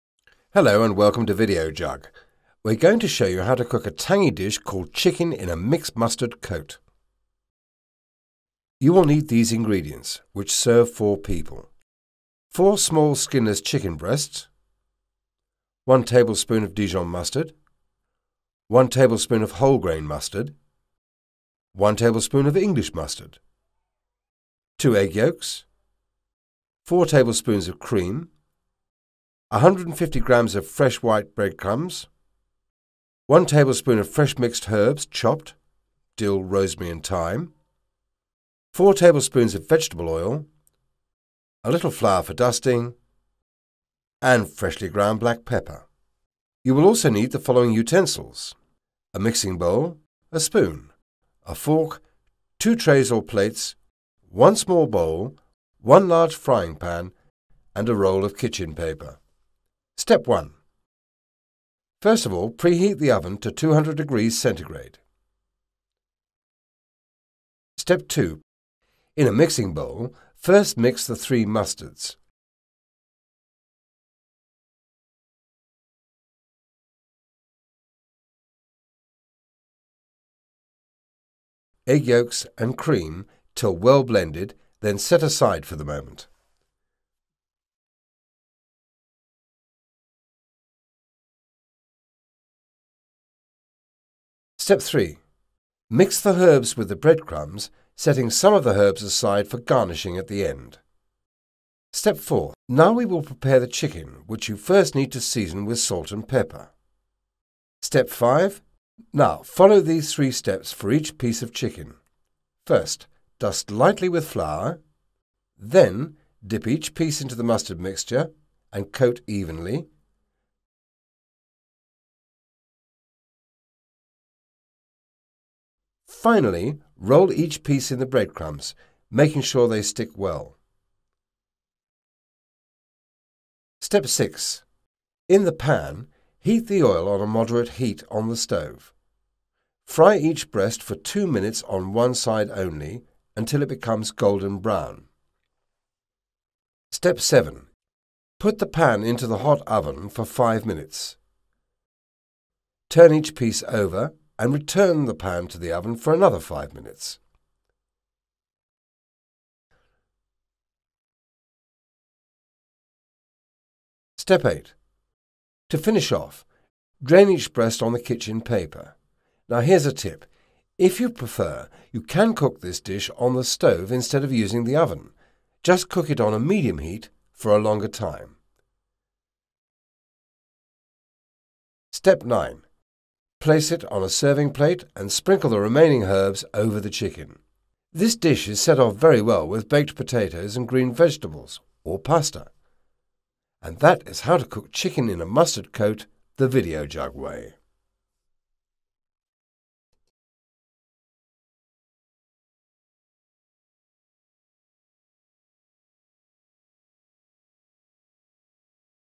How To Make Chicken In A Mustard Coat
Chicken In A Mustard Coat Recipe. This tangy chicken dish makes a perfect change to what you can do with that everyday bird. It goes well with vegetables, potatoes, and pasta. Savour our Chicken In A Mustard Coat recipe.